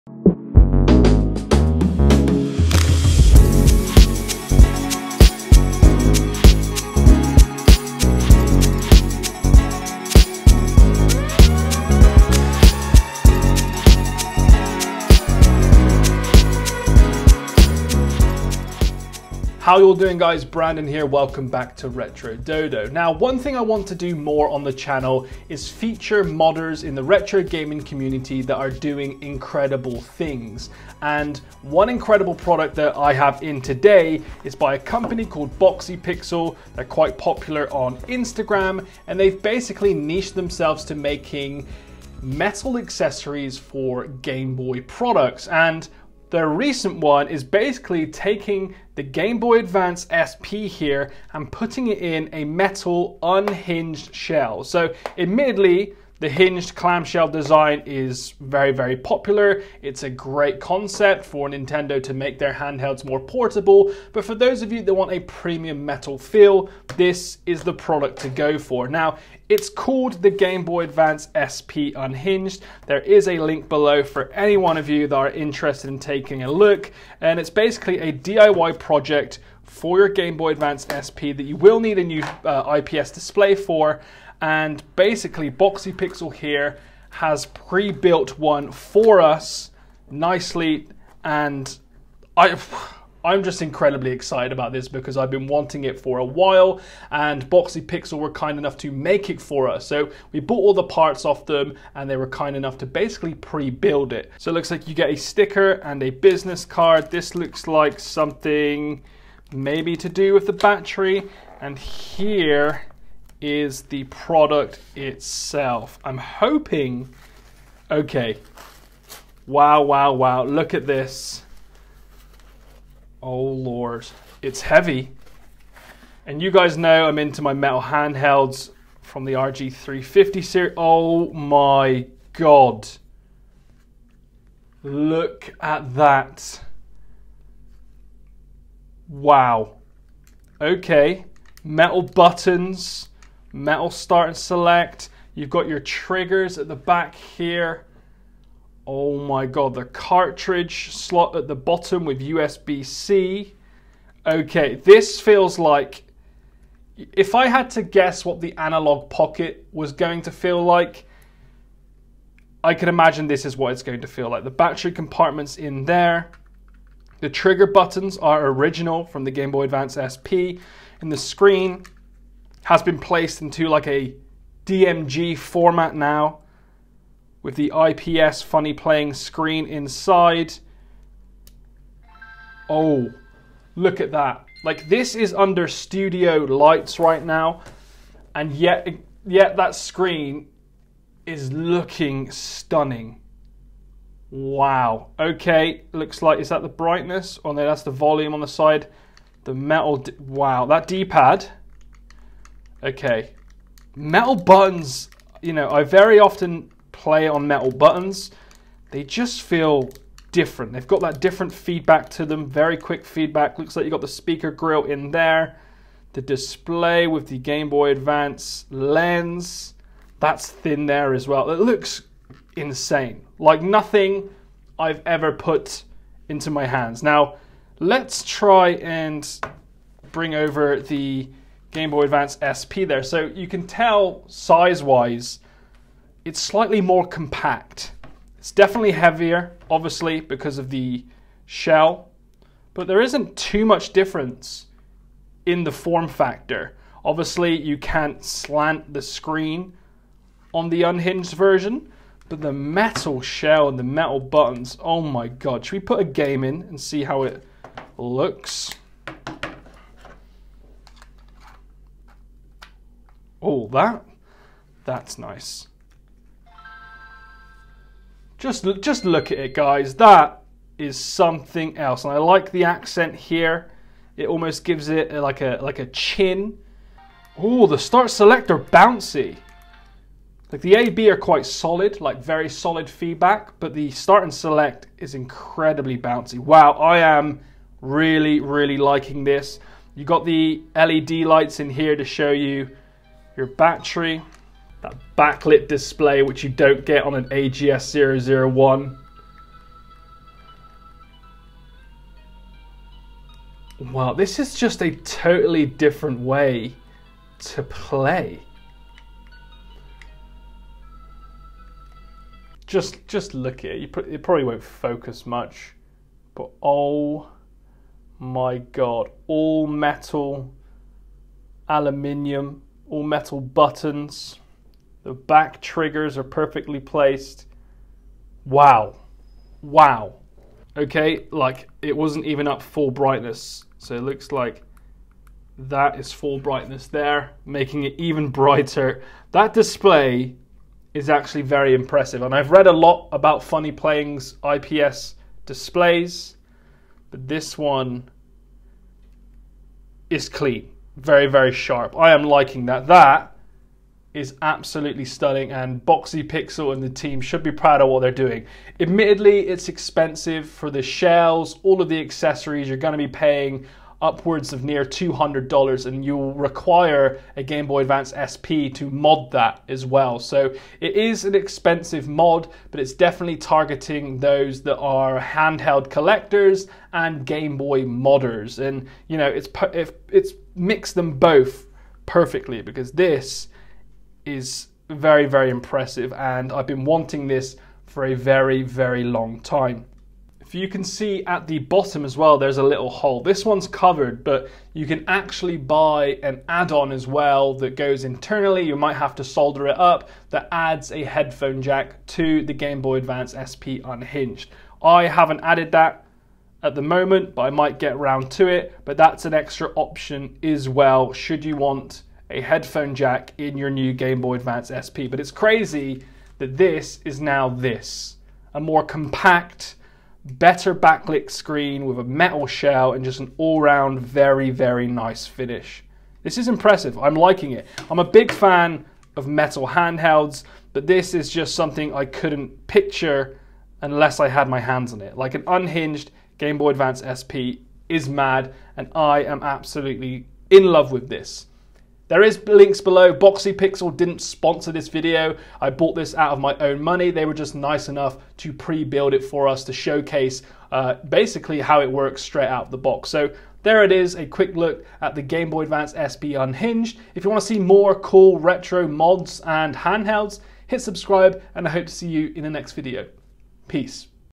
Unboxing The ULTIMATE Metal Gameboy Advance SP Mod
We take a look at the Boxy Pixel Unhinged Gameboy Advance SP, and what we're calling one of the best GBA SP mods we've ever seen!

💻 CHAPTERS 💻
00:00 Introduction
01:20 Unboxing
02:11 First Impressions
03:50 Gameboy Advance SP Comparison
04:45 Gameplay
05:13 Final Impressions